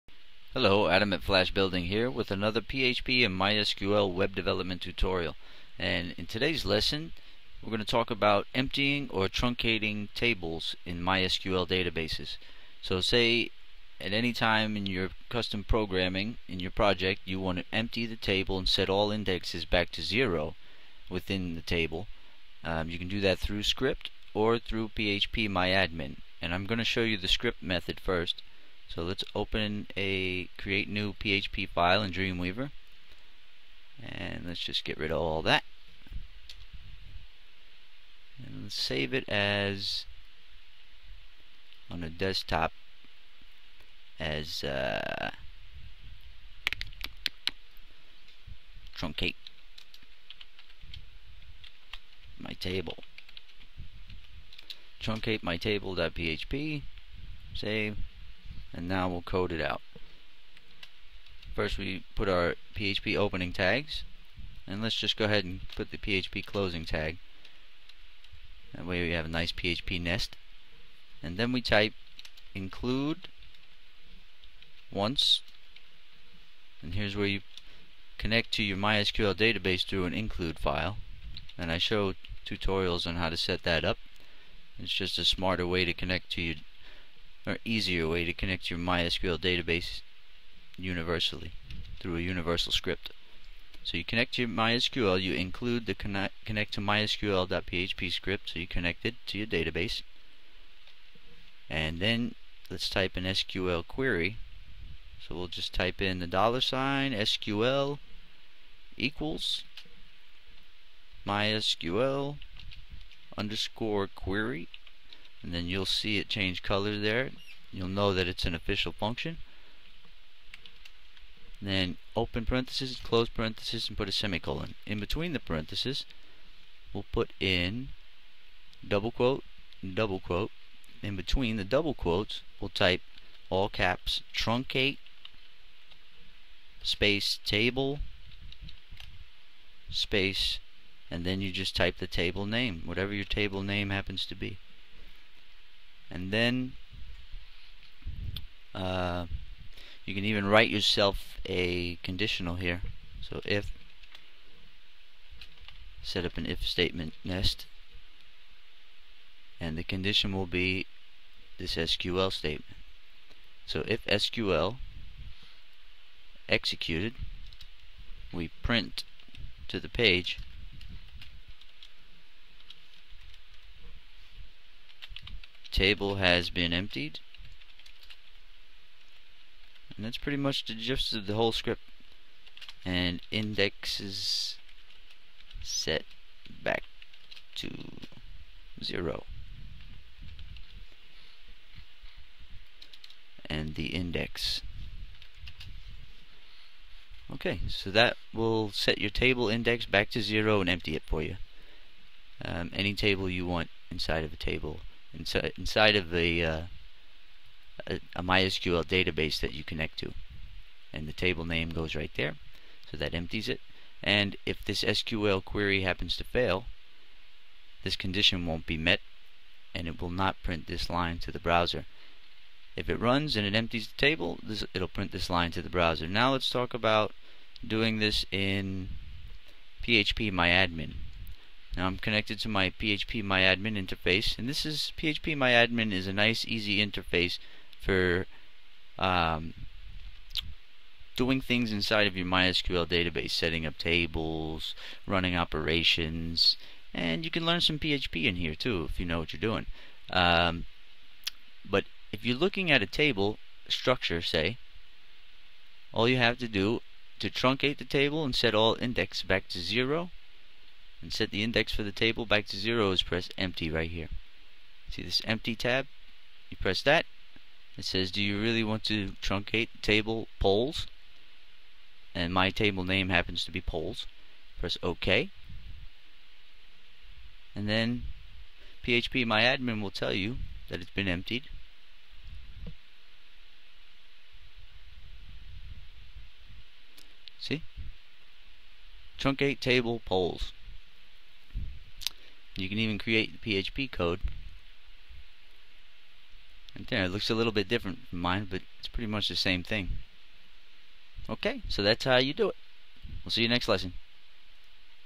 How to Empty or Truncate Your MySQL Database Table Tutorial
Learn how to empty or truncate MySQL database tables through script, or manually using PHPMyAdmin. Sets indexes back to zero as well.